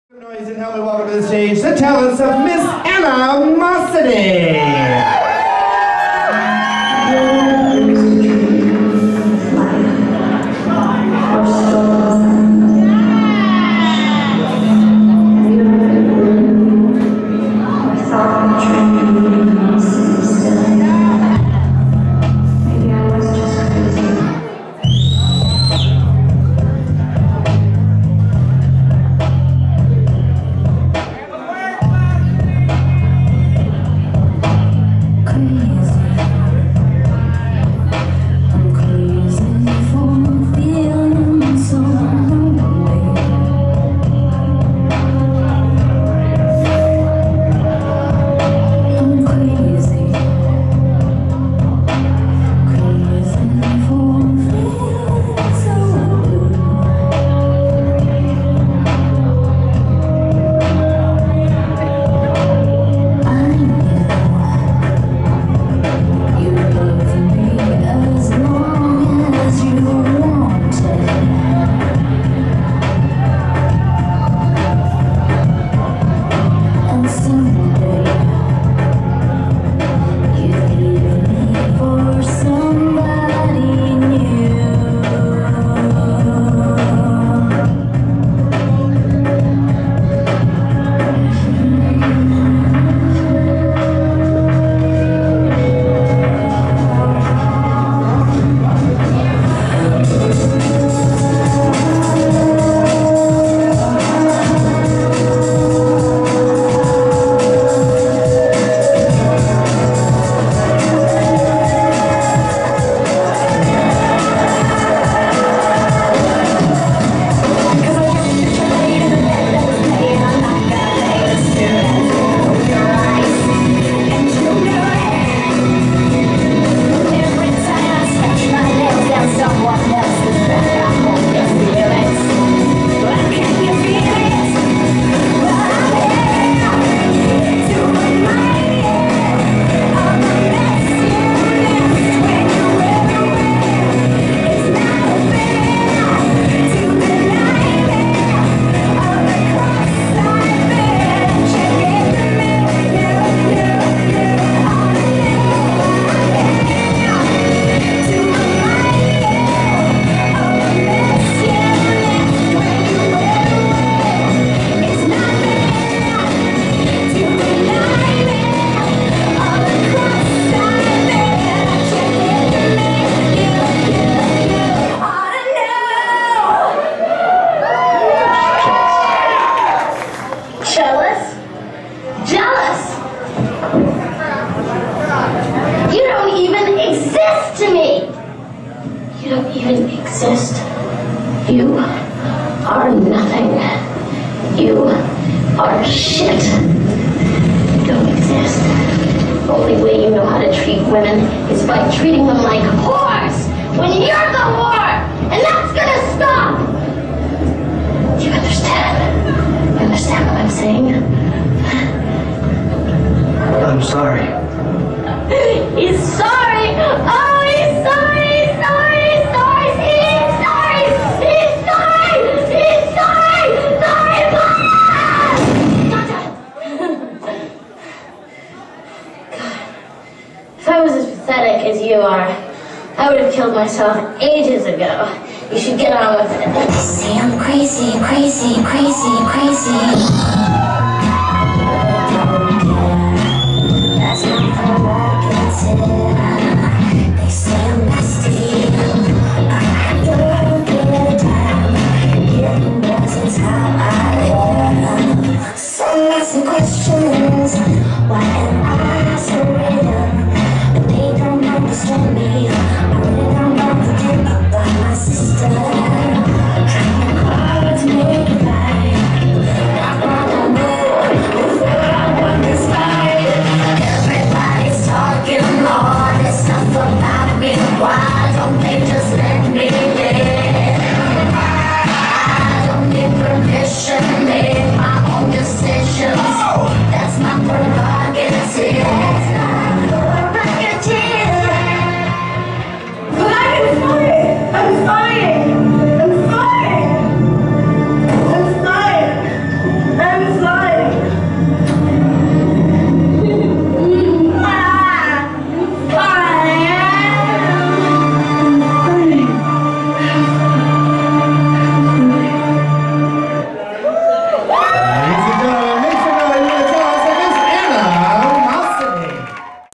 Miss Amateur PowerDiva - Anna Mossity 1
Nine contestants compete for the titles of Miss Amateur PowerDiva and PowerDiva Newcomer. Miss Amateur PowerDiva Sarina Le Blonc-Sanchez and PowerDiva Newcomer Armani Evangelista will be handing down their titles. Hosted by Nickki Stevens with performances from Deja Dellataro, Dice Santana, Anna Mossity and Eva Angelica. Filmed at Rumors Nightclub and filmed by Daydream Studios.